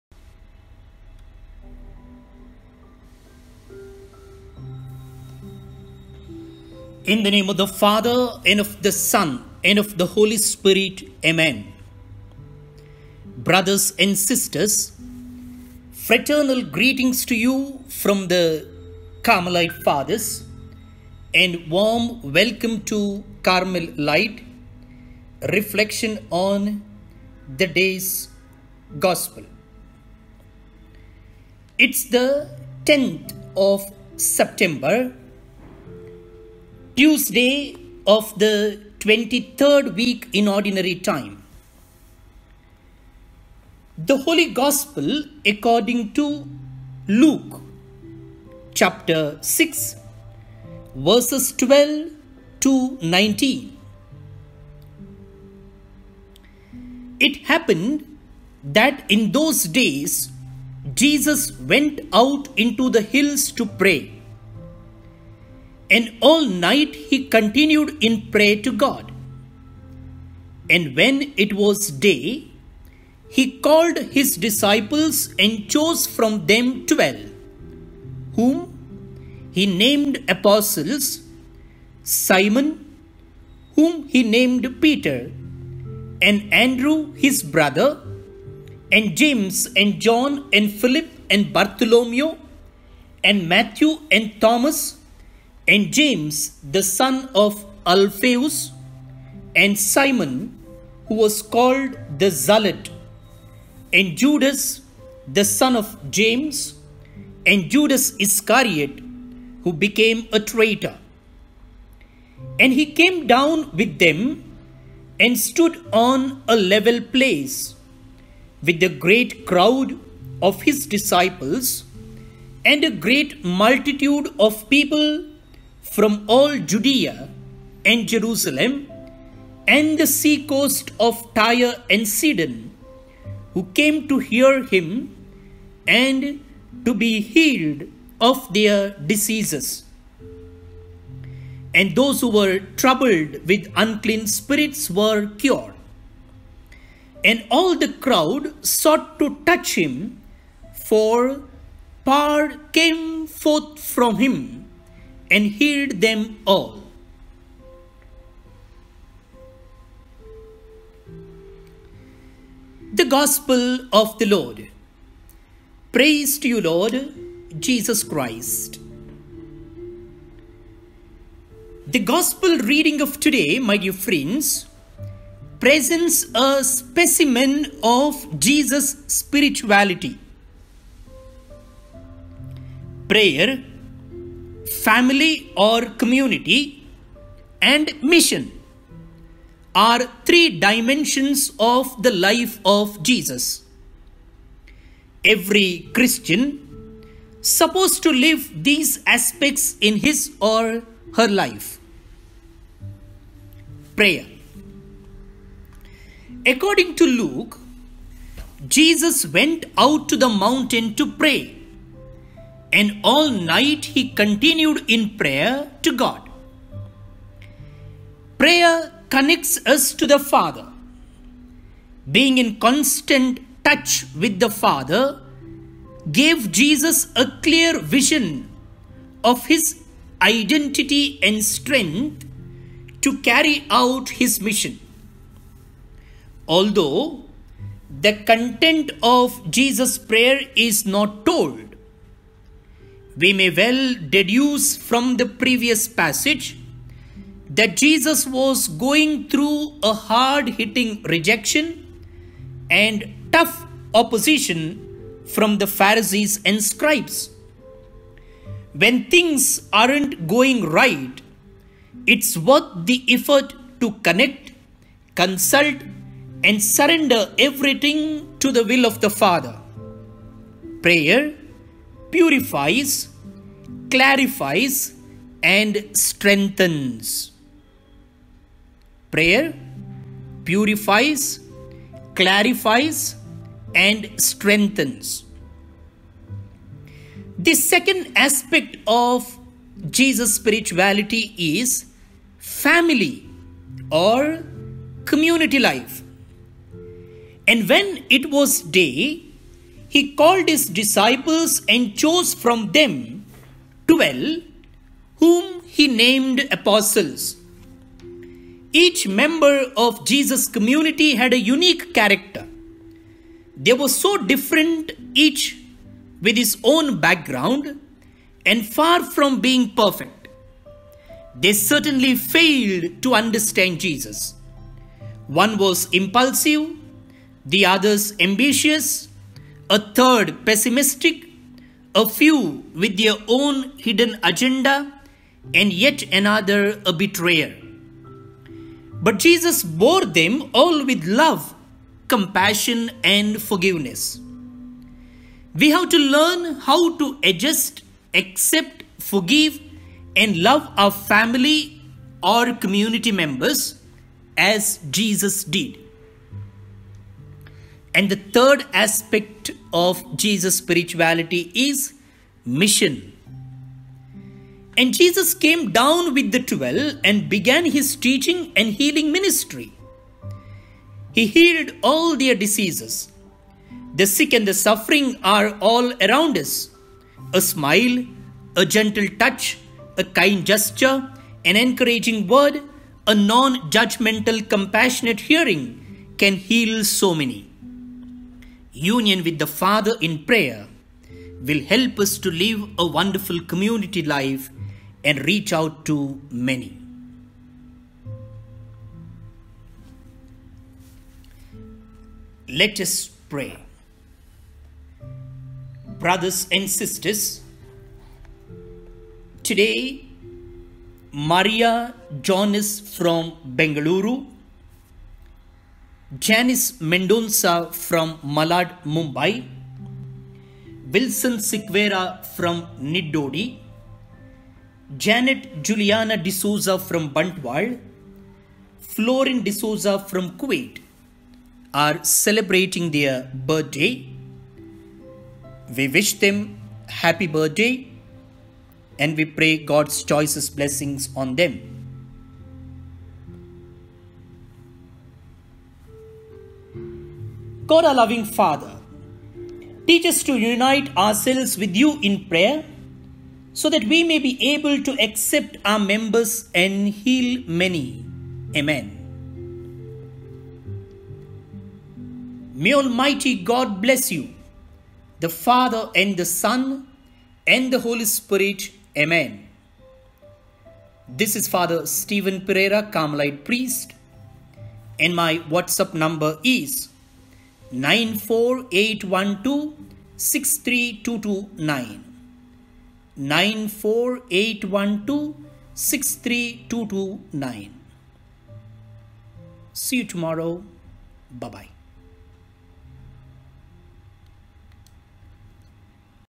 CARMEL LIGHT I Prayer, Family and Mission I Sept. 10 I Twenty Third Tuesday I Luke 6: 12-19
CARMEL LIGHT is a daily reflection on the Word of God by Rev Fr Stifan Perera, a Carmelite priest from Infant Jesus Shrine, Carmel Hill - Mangalore, Karnataka – INDIA